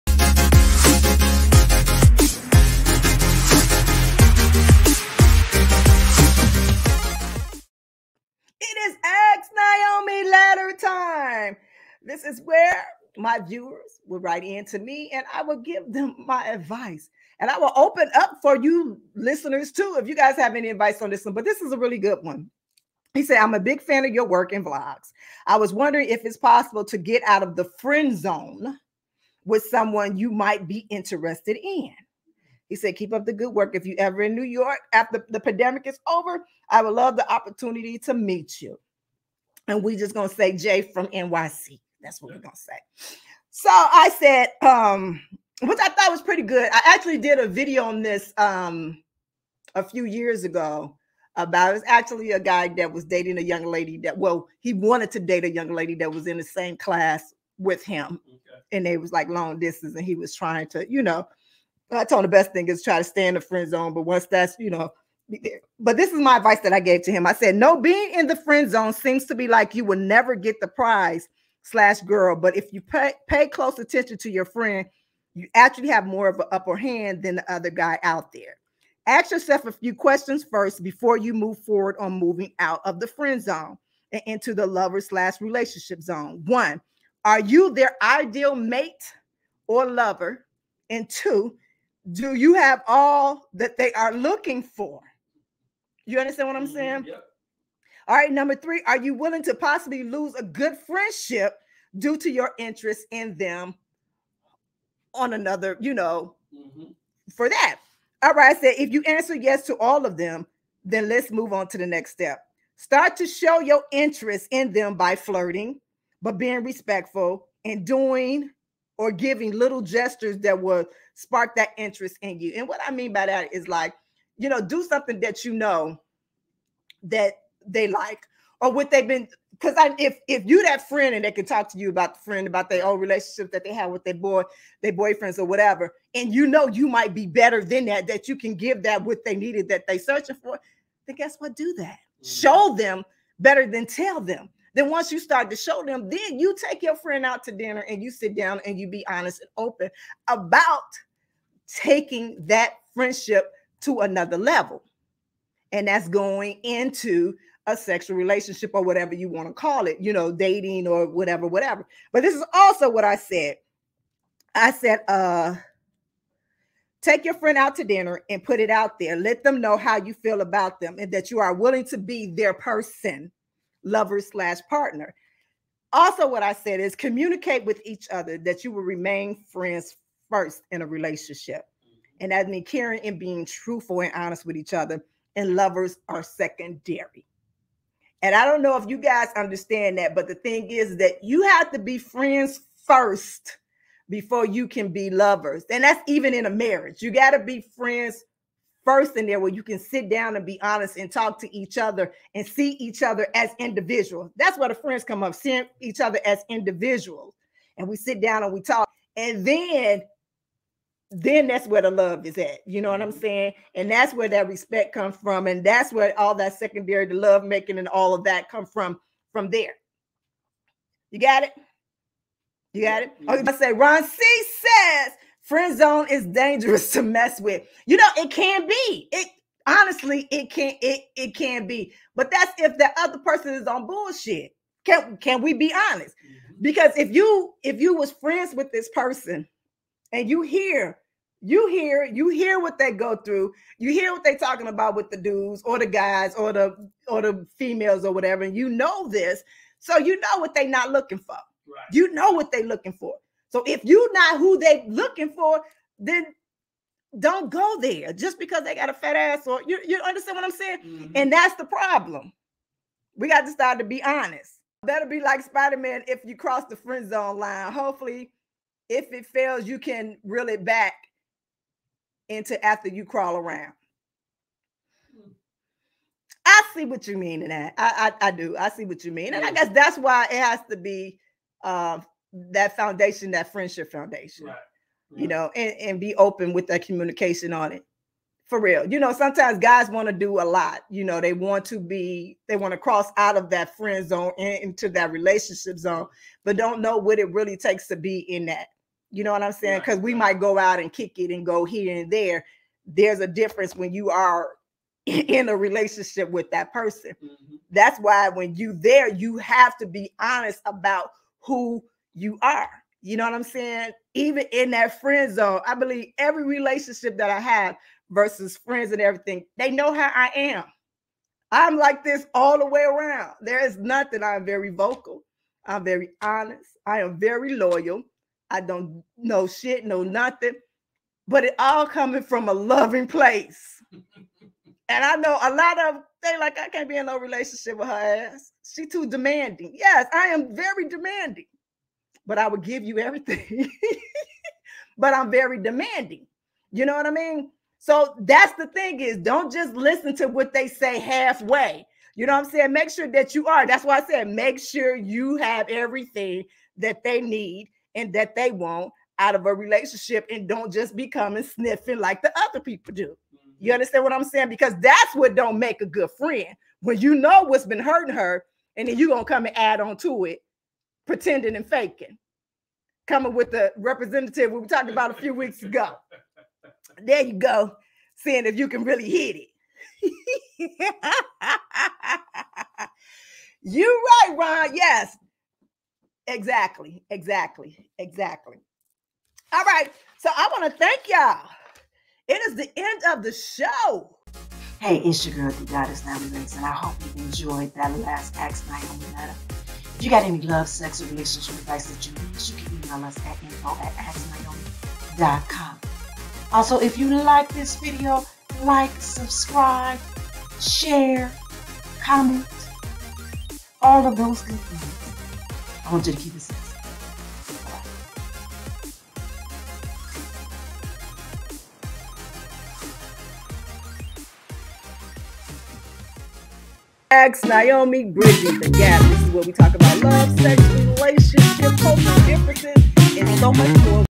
ASK NYOMI LETTTER "HOW TO GET OUT THE FRIEND ZONE"
HOW TO GET OUT OF THE FRIEND ZONE AND INTO THE LOVERS ZONE WS THE ADVICE LETTER THIS WEEK ON ASK NYOMI. NYOMI WITH THE HELP OF DOG POUND MIKE MIKE AND THE LIVE VIEWERS GAVE  ADVICE TO HIM ON HOW TO GET OUT OF THE FRIEND ZONE AND INTO THE LOVERS ZONE.